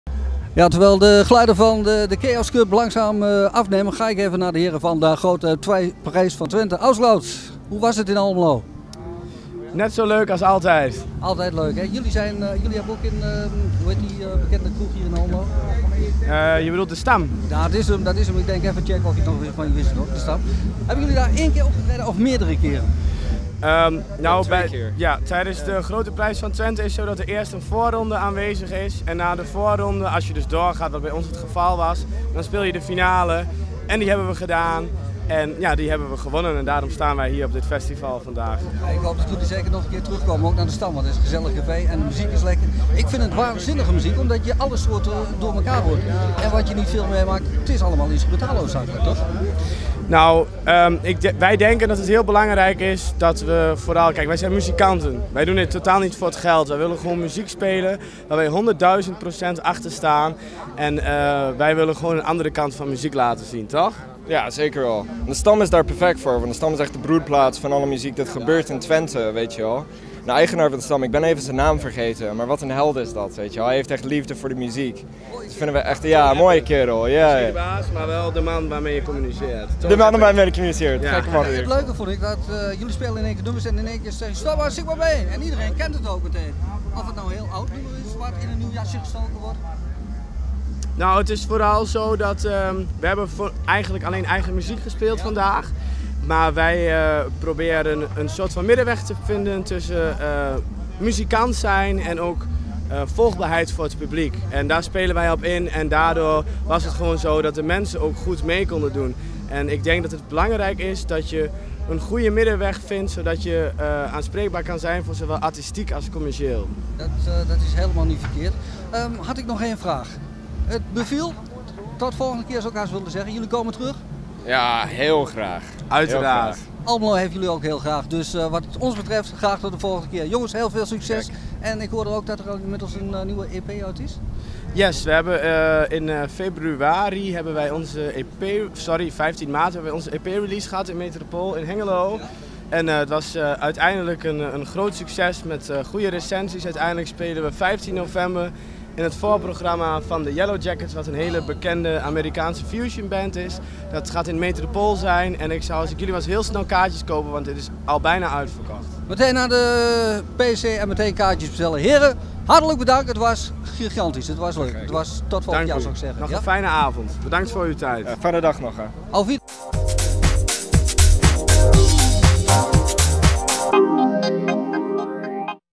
Interview met Joey Vos en Noah Ralph van AUFGLAUFT bij Hobnob 2018
Door de verschillende muziekstijlen was Aufglauft lekker bezig op Hobnob en hebben dus niet zomaar  De Grote Prijs van Twente gewonnen.